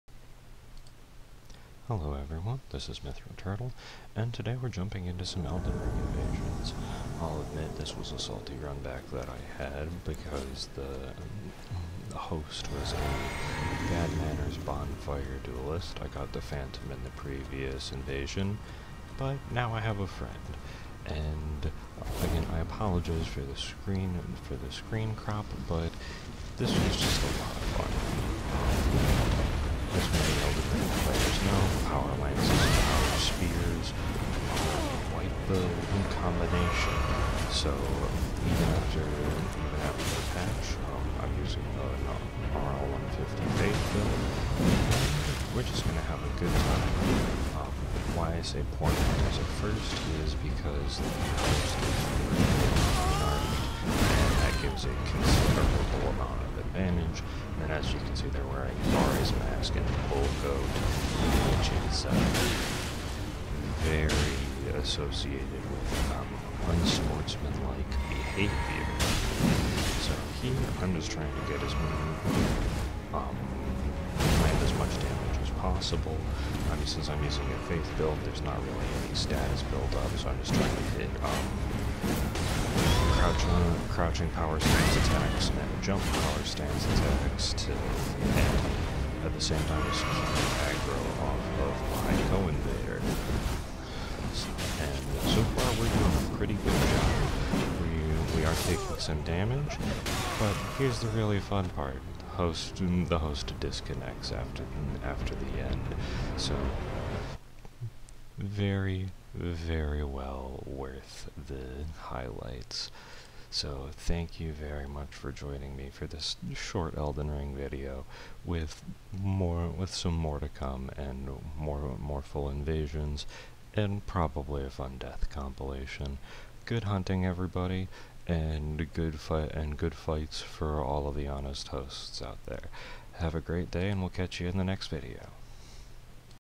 Bonfire Duelist Disconnects After Loss
Re-invading a bonfire duelist with poor manners, but this time with a friend! Sorry for the poor sound balance and cropped screen. I still hope you enjoy the revenge highlight.

Build is RL150 strength and faith.